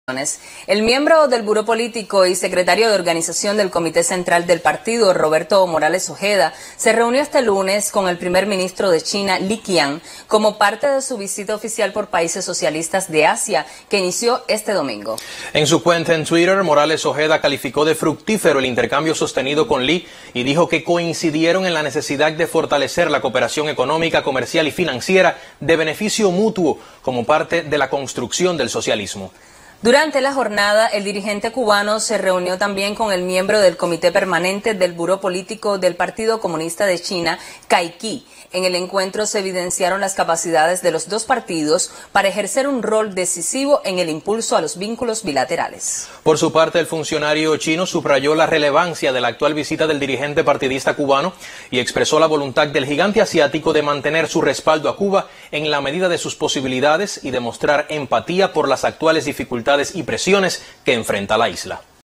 Recibió Primer Ministro de China a Roberto Morales Ojeda
El miembro del Buró Político y Secretario de Organización del Comité Central del Partido de Cuba, Roberto Morales Ojeda, se reunió este lunes con el primer ministro de China, Li Qiang, como parte de su visita oficial por países socialistas de Asia, que inició este domingo. (Tomado del Sistema Informativo de la Televisión Cubana)